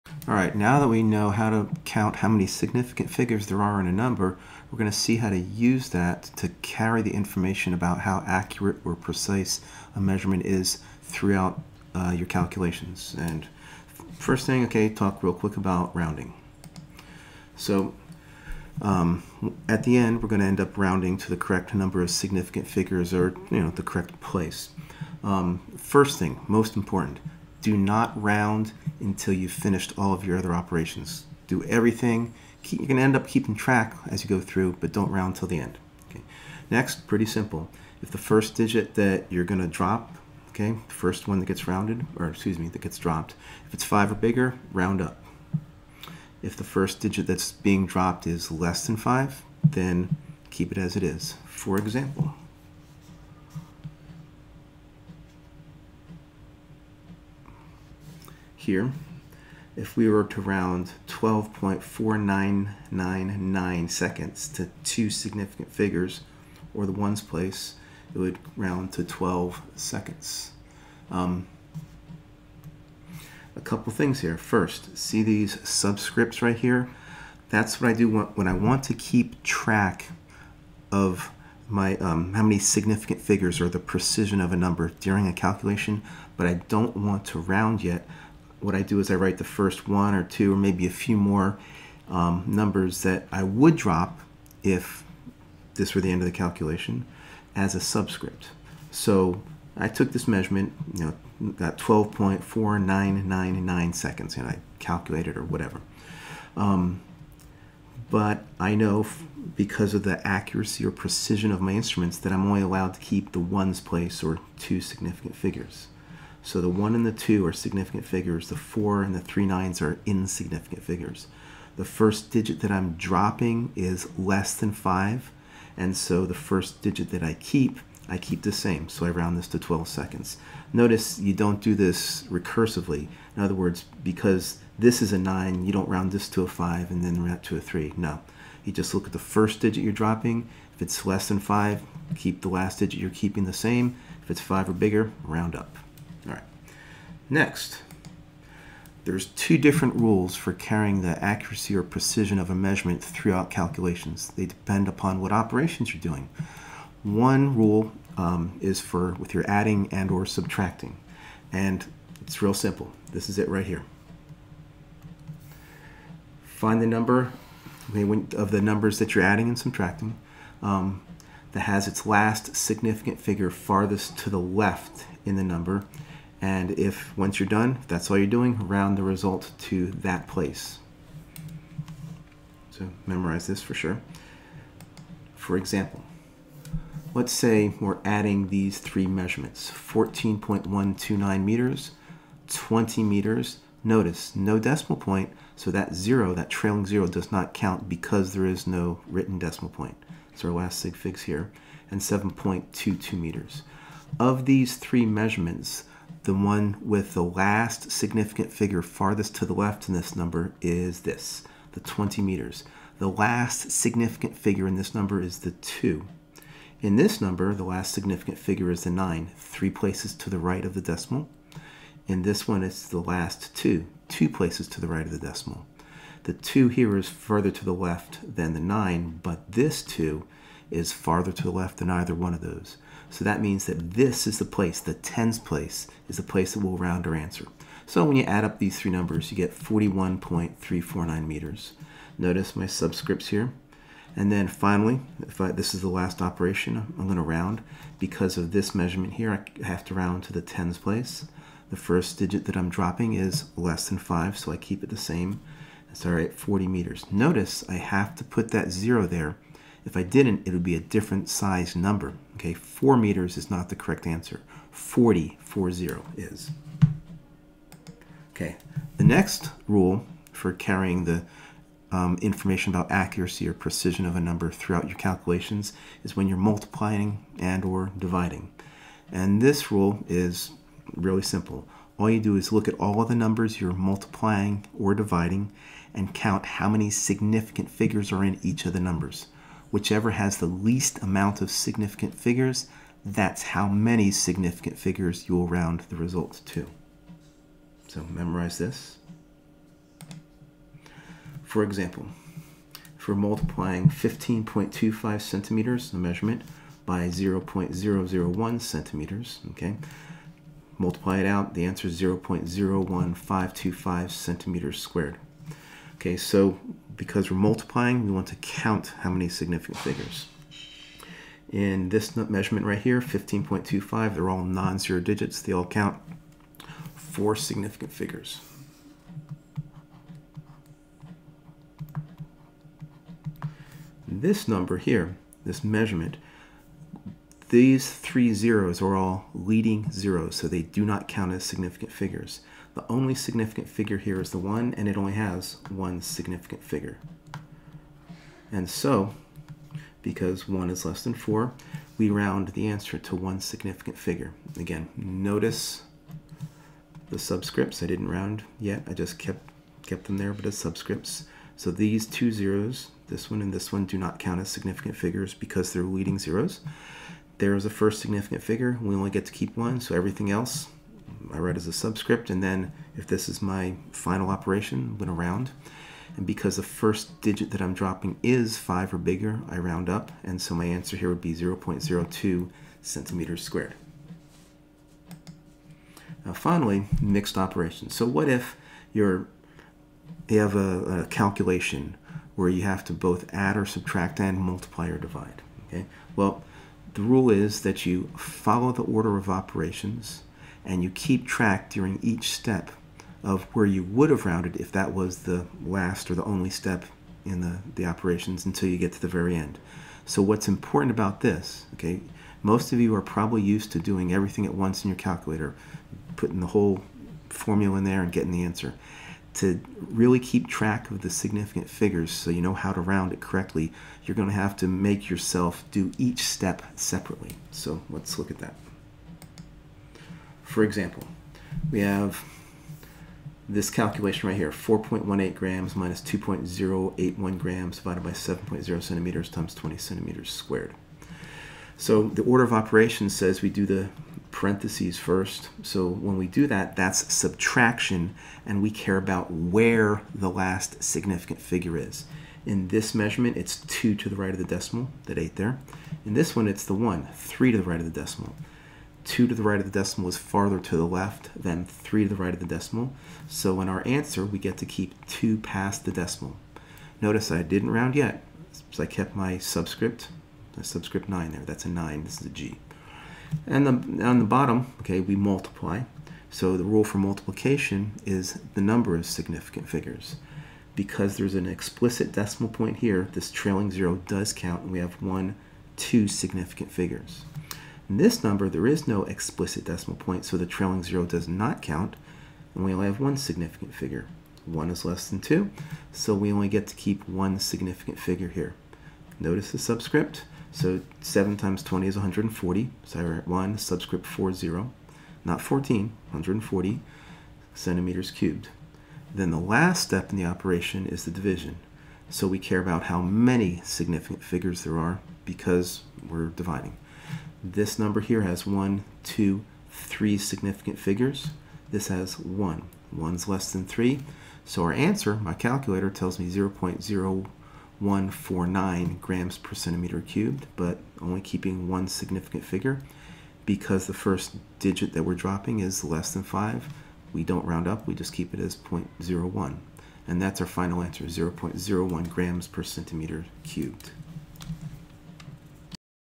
Chem101SignificantFiguresRoundinginOperations
Rules for rounding to the correct number of significant figures during addition, subtraction, multiplication, and division.